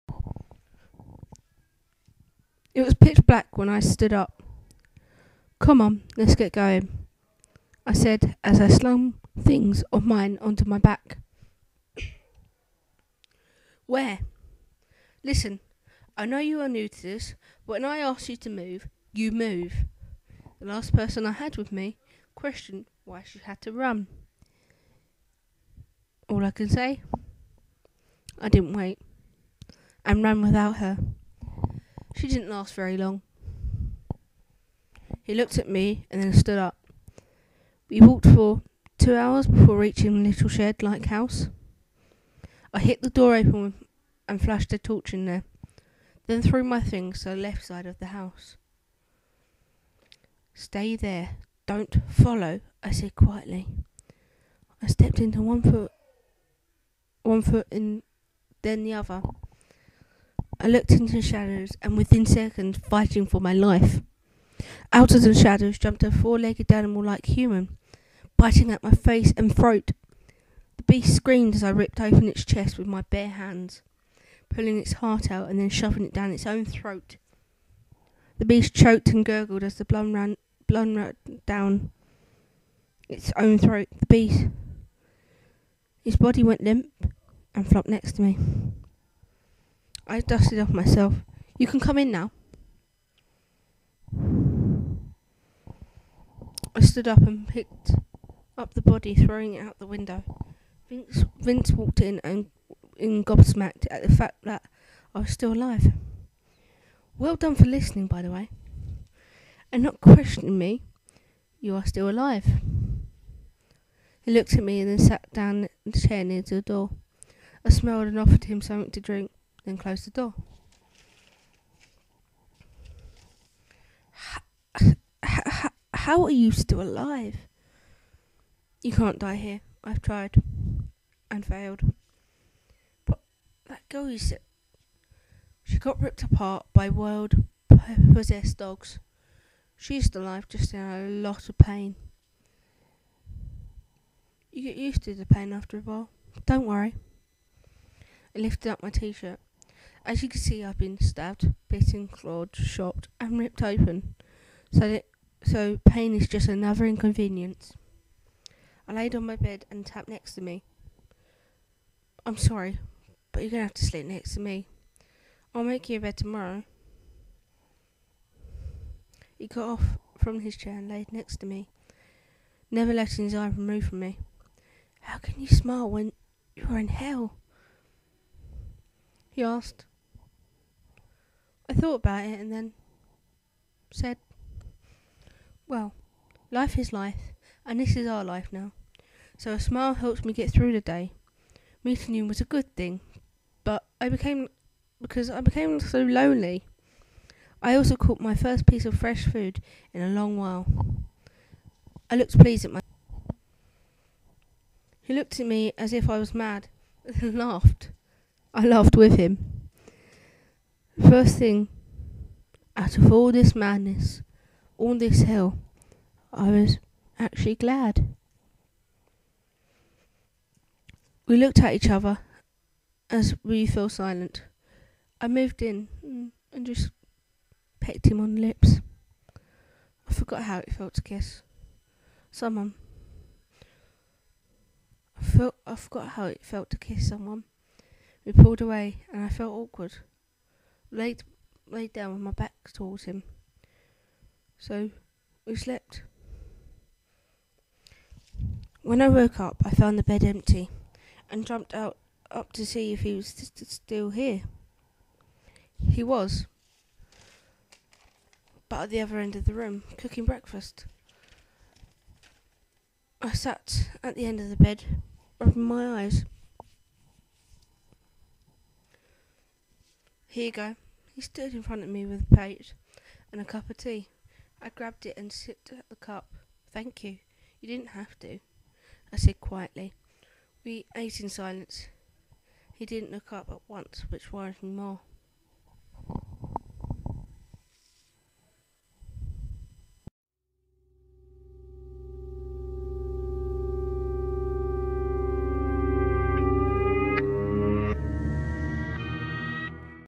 the dark shadow part 2
Emily finds her self stuck with this stranger good or bad who knows but what every happens nothing can come out good. part three will be on tomorrow